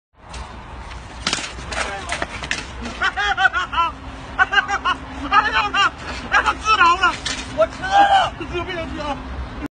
When everything made in China 😂😂😂
Two man tried to ride a bicycle and this is what happened.